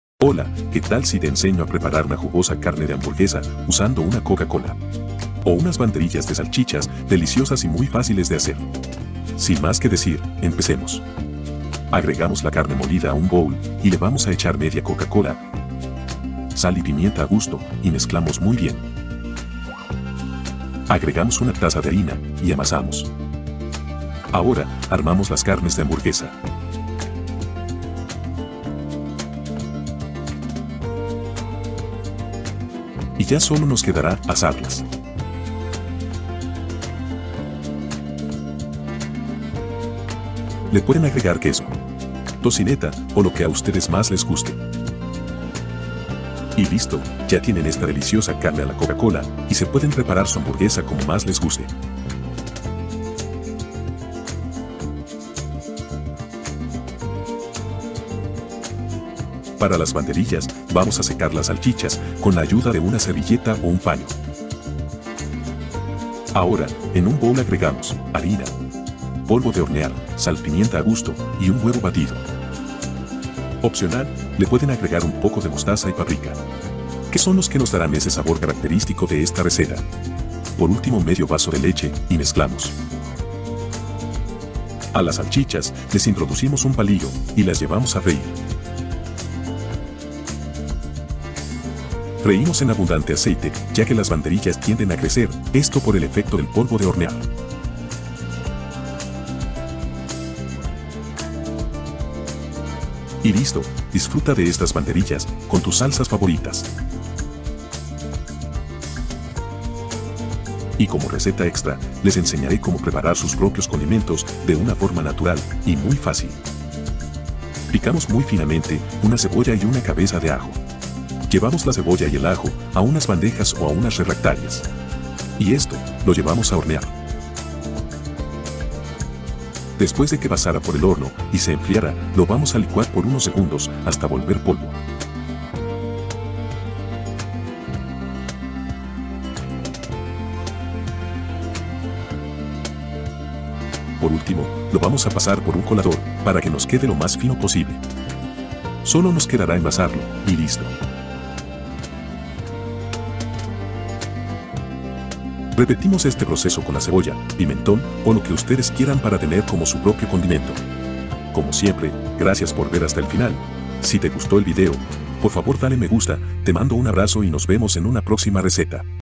¡No Conocías Esta Hamburguesa de Coca-Cola! o Banderillas Caseras - Jugosa Carne que te Sorprenderá
Ya probaste estas deliciosas banderillas o esta carne a la coca-cola. Disfrútala y sorprende a tus invitados.